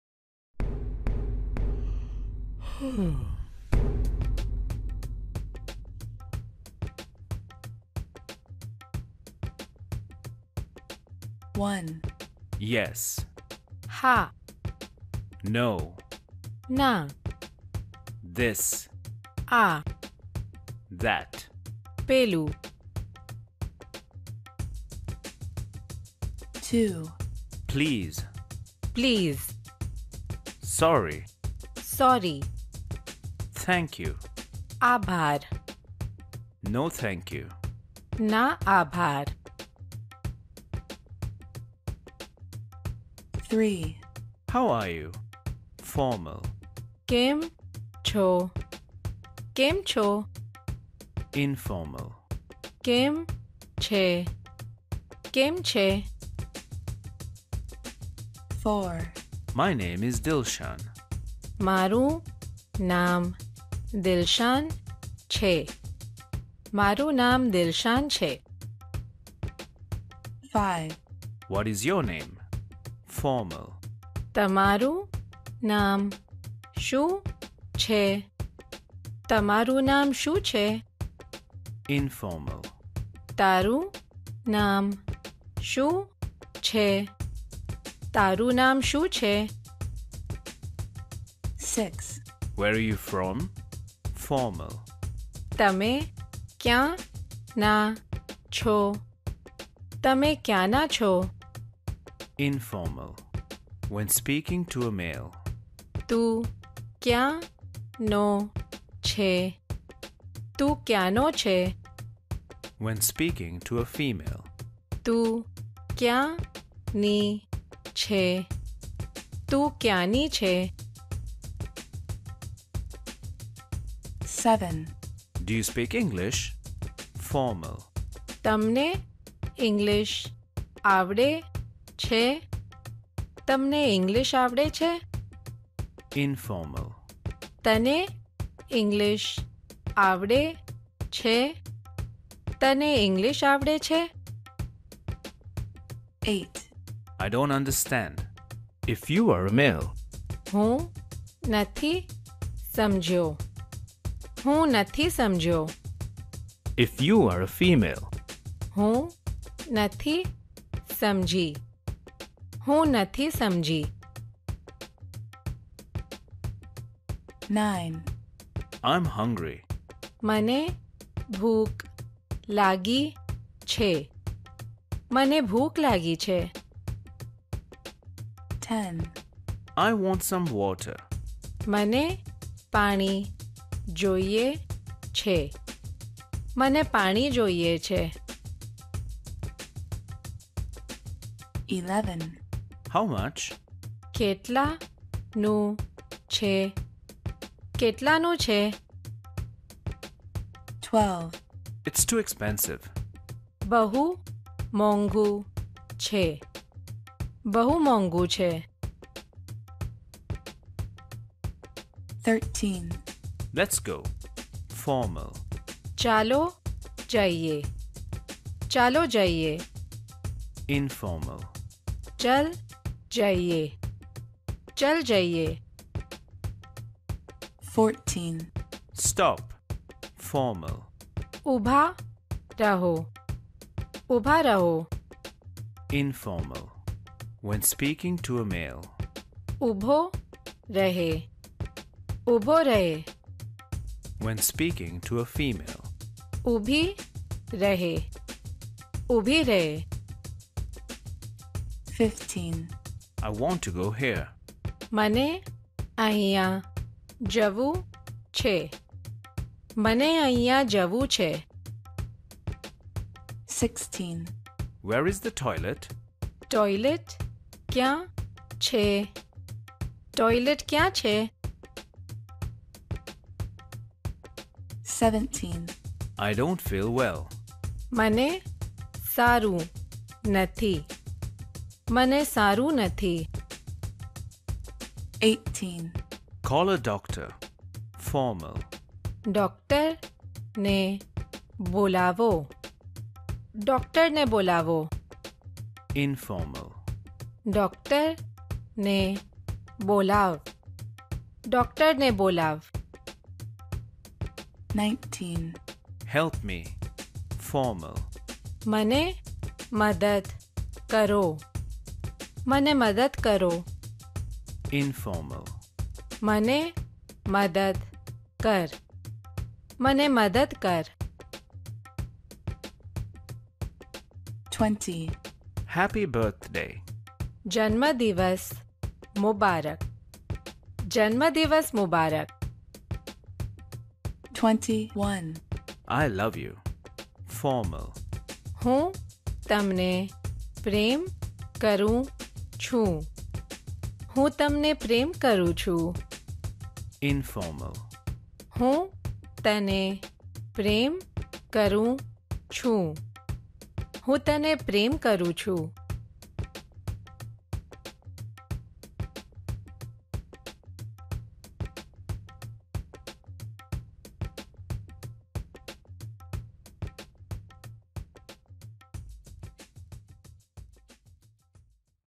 Learn Gujarati Language: 21 Gujarati Phrases You Absolutely Must Know! (+ free phrasebook)
See you on the blog. Soon.

Dilshan

learn Gujarati, Gujarati phrases, Gujarati lessons, speak Gujarati, Gujarati words, Gujarati tutorials, Gujarati language

List of Gujarati words you'll learn in this Learn Gujarati Video:
- Learn Gujarati language word for “Yes”
- Learn Gujarati language word for “No”
- Learn Gujarati language word for “This”
- Learn Gujarati language word for “That”
- Learn Gujarati language word for “Please”
- Learn Gujarati language word for “Sorry”
- Learn Gujarati language word for “Thank you”
- Learn Gujarati language word for “No, thank you”

List of Gujarati phrases & sentences you'll learn in this Learn Gujarati Video:
- Learn Gujarati language phrase for “My name is”
- Learn Gujarati language phrase for “What is your name?”
- Learn Gujarati language phrase for “Where are you from?”
- Learn Gujarati language phrase for “How are you?”
- Learn Gujarati language phrase for “Do you speak (know) English?”
- Learn Gujarati language phrase for “I don’t understand”
- Learn Gujarati language phrase for “I’m hungry”
- Learn Gujarati language phrase for “I want (some) water”
- Learn Gujarati language phrase for “How much?”
- Learn Gujarati language phrase for “(It’s) too expensive”
- Learn Gujarati language phrase for “Let’s go”
- Learn Gujarati language phrase for “Stop”
- Learn Gujarati language phrase for “I want to go here”
- Learn Gujarati language phrase for “Where is the toilet?”
- Learn Gujarati language phrase for “I don’t feel well”
- Learn Gujarati language phrase for “Call a doctor!”
- Learn Gujarati language phrase for “Help me!”
- Learn Gujarati language phrase for “Happy birthday!”
- Learn Gujarati language phrase for “I love you”